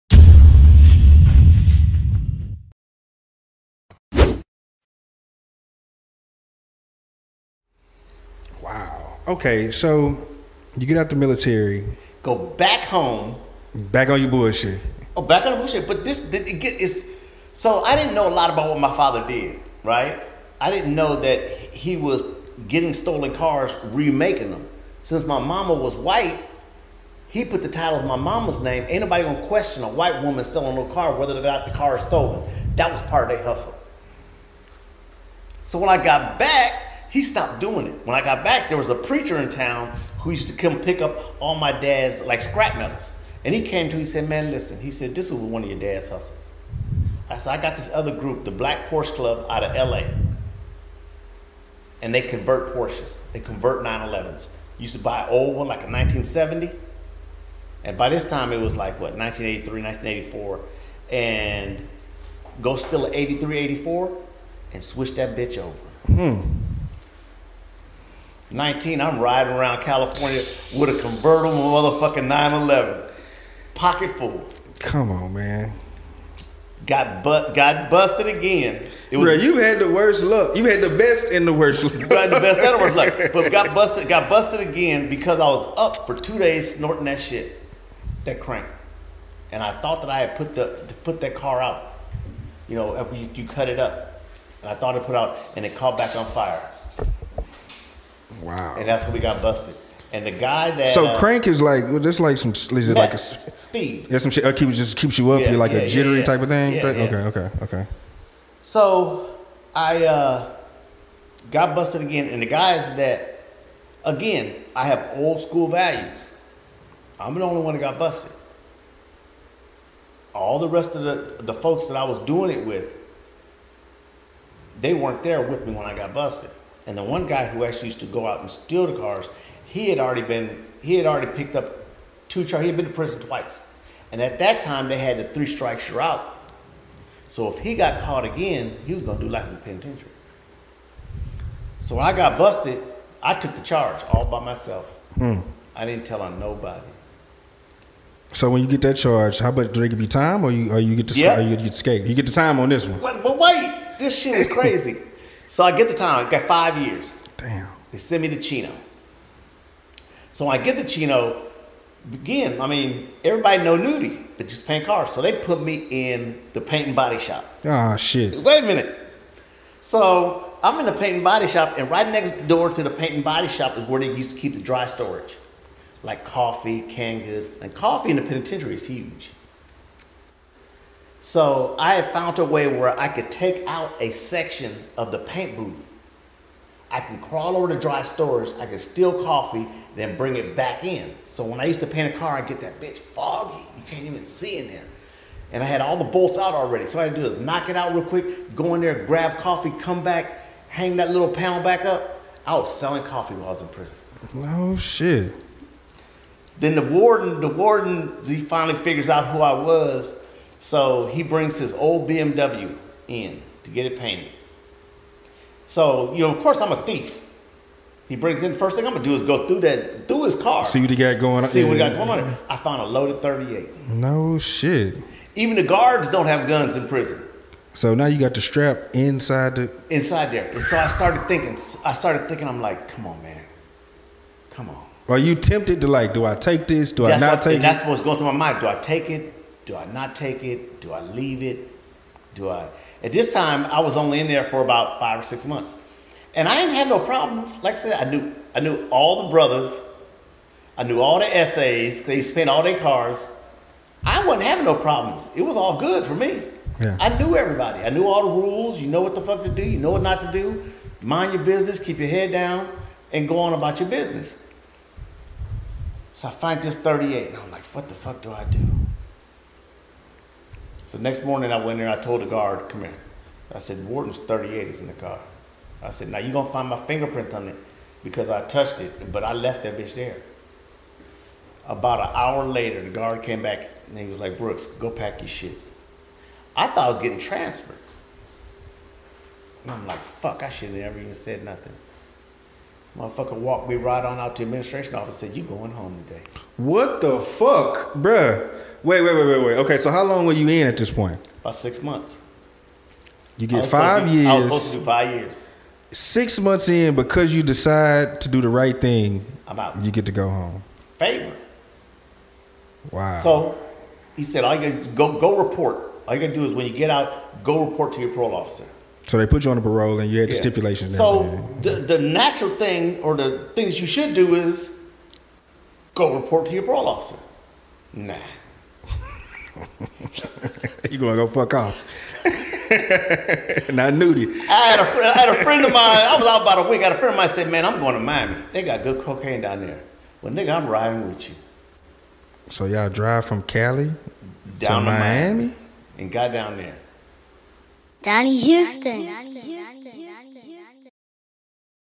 Eugene Brooks: Going Back to Prison, Only Doing 6 months on a 5 Year Sentence (Part 3)
Eugene tells Donnie Houston about how he ended up back in prison after returning from the military but was able to leave early for doing the right thing.